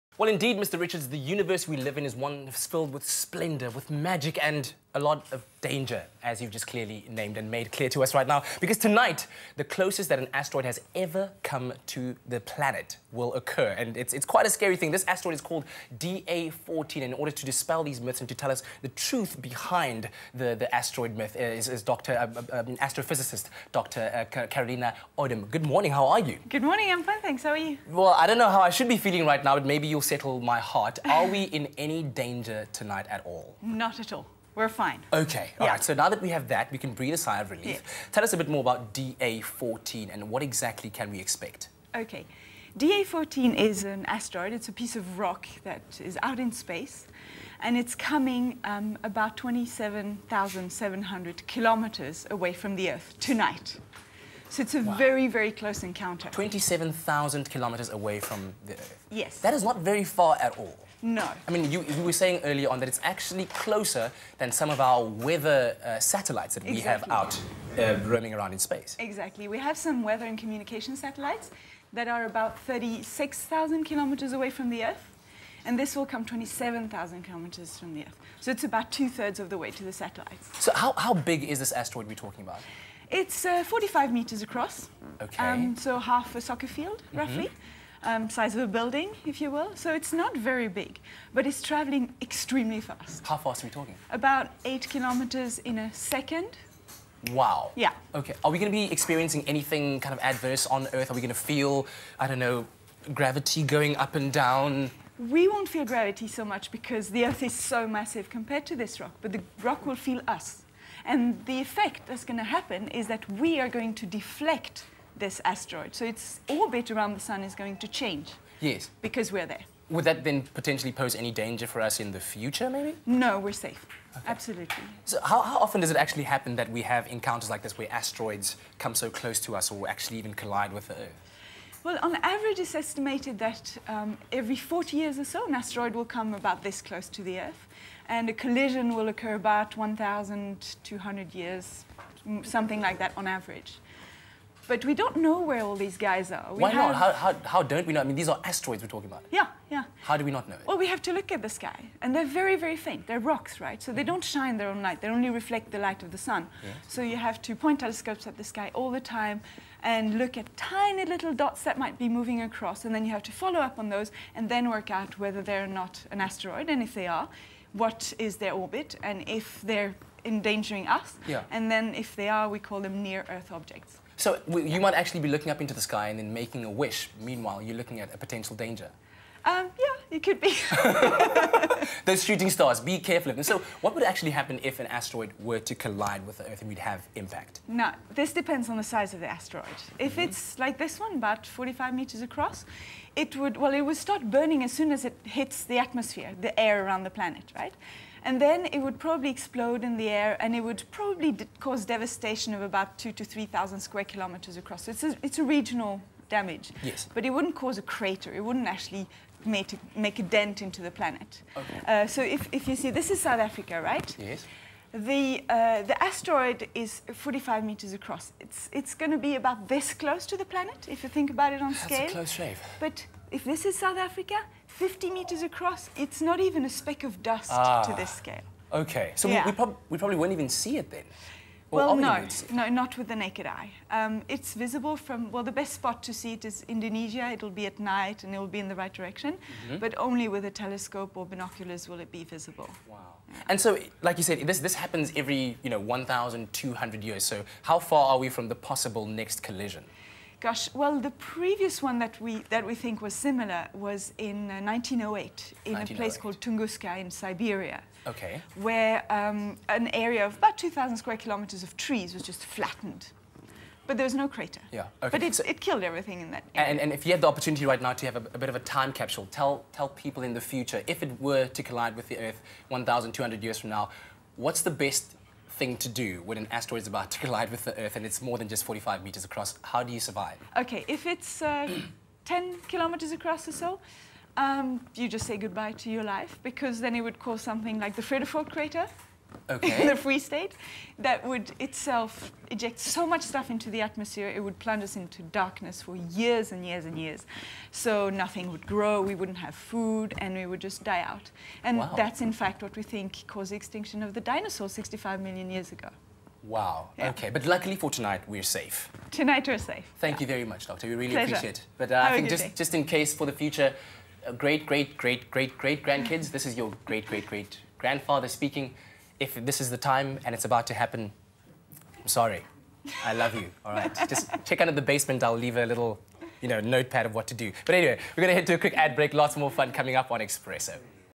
Asteroid DA-14 (15.2.2103)
Dr Carolina Ödman will chat about the asteroid DA-14's proximity to earth, size and the risks and consequences of asteroids hitting earth.